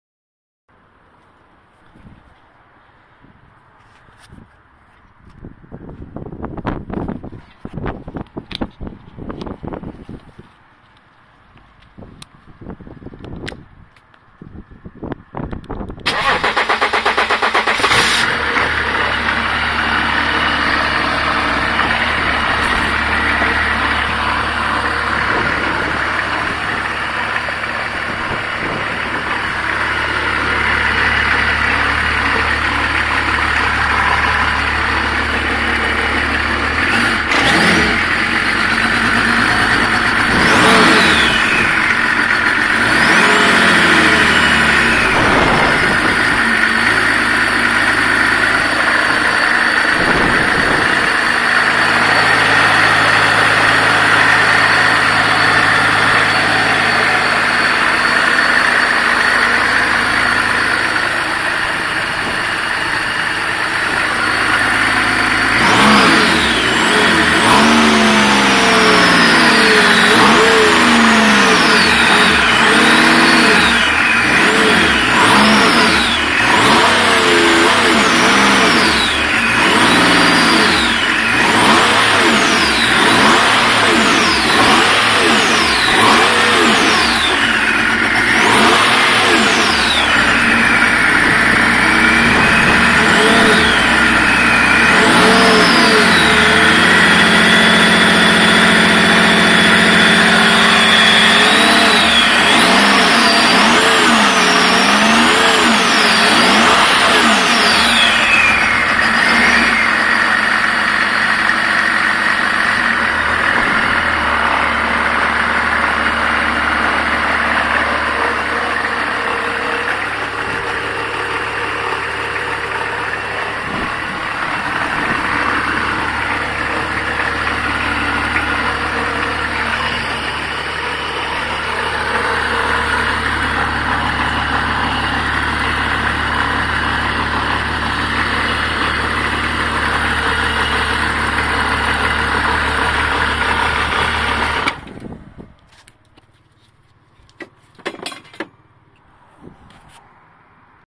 BMW M51 Engine Test (From Omega 2.5td x25dt)
Testing the BMW M51 6 Cylinder Turbo Diesel engine. This was removed from a Vauxhall Omega with about 147,000 miles on the clock. I have used the cars wiring /ecu / immobiliser. The plan was to fit a pair of the engines in my boat. These engine are found in 325TDS and 525TDS BMWs, Omegas, and Range Rovers.